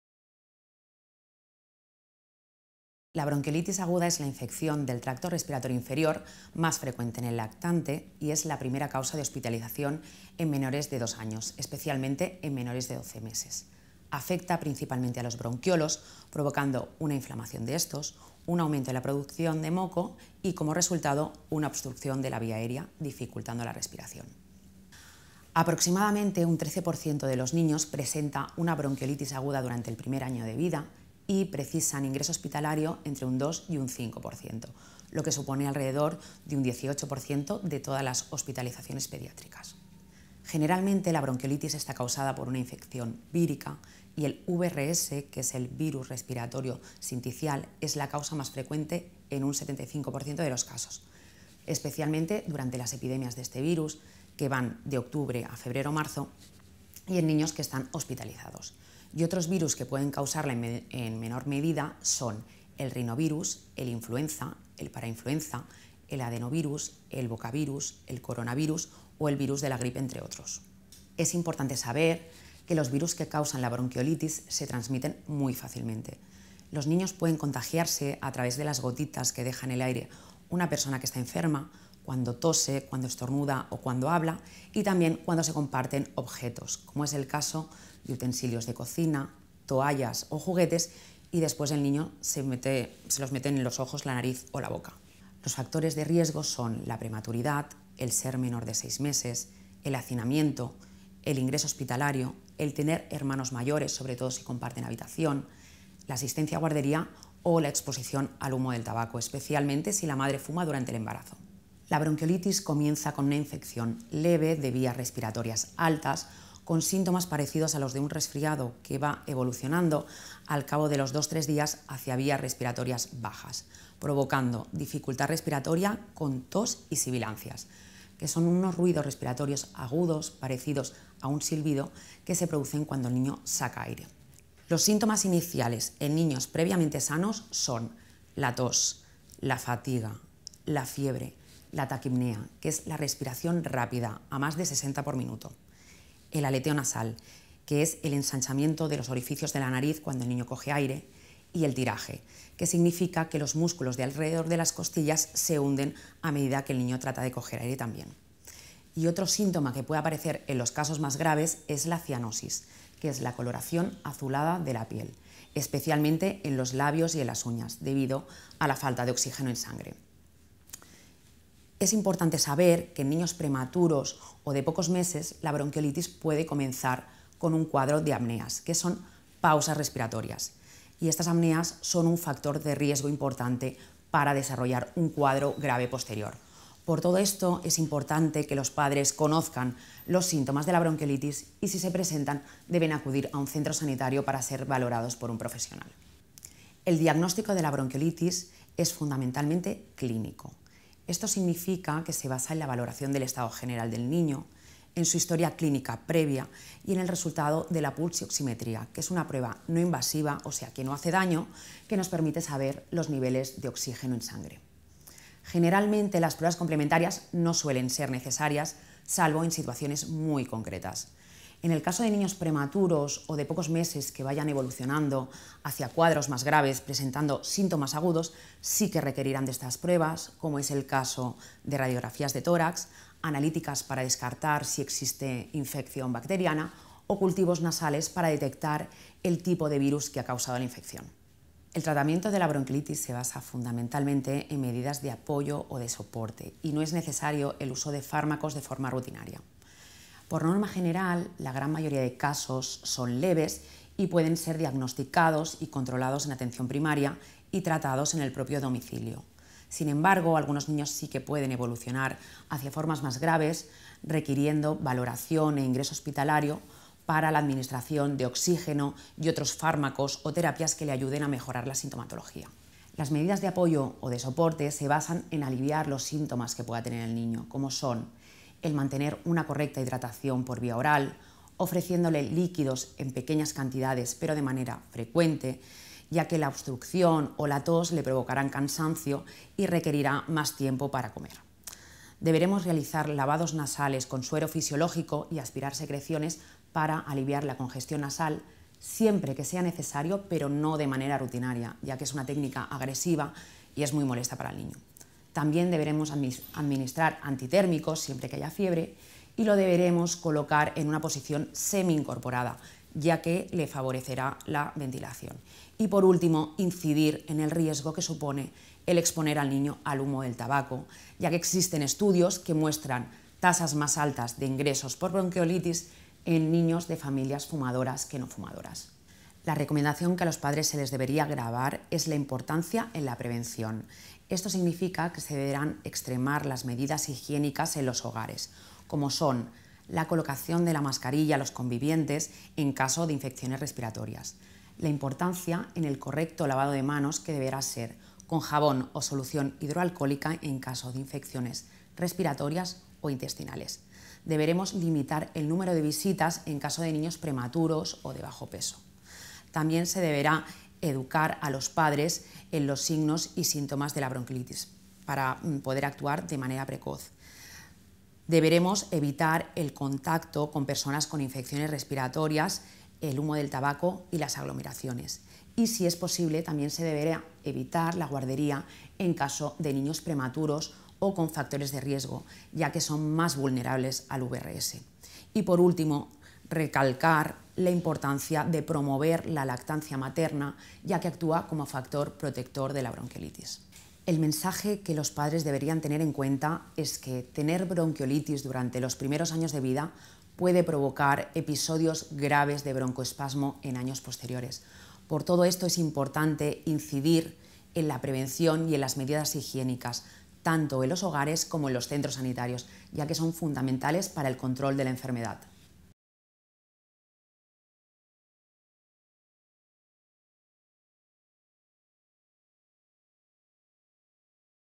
Bronquiolitis del prematuro | PortalCLÍNIC
La bronquiolitis aguda es la infección del tracto respiratorio inferior más frecuente en el lactante y es la primera causa de hospitalización en menores de dos años. Afecta principalmente a los bronquiolos, provocando una inflamación de éstos, un aumento en la producción de moco y como resultado una obstrucción de la vía aérea, dificultando la respiración.